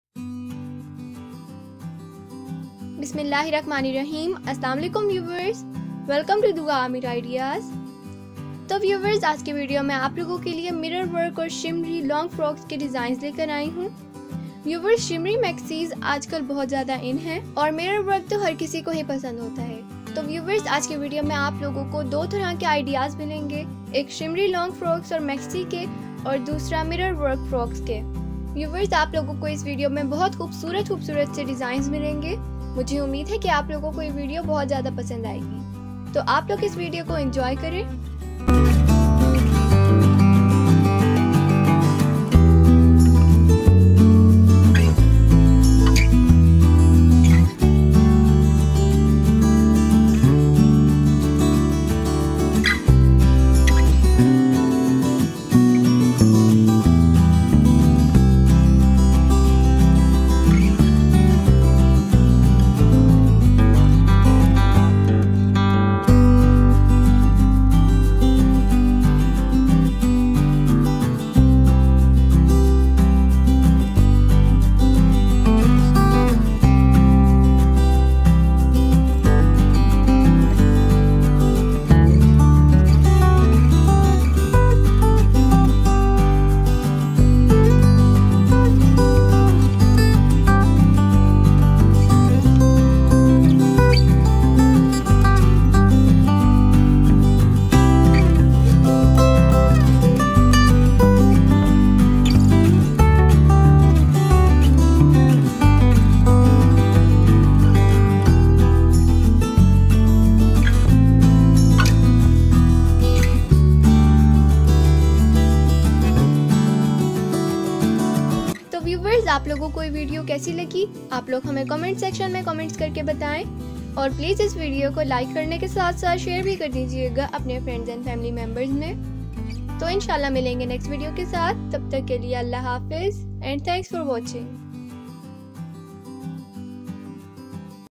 Mirror Work & Shimmery Long Frocks design
Assalam-o-Alaikum!
Hope that you guys are well.
So in today's video I will show you
Beautiful Mirror and Shimmery Long Frocks Design.
I hope you guys will like this video.
Thanks!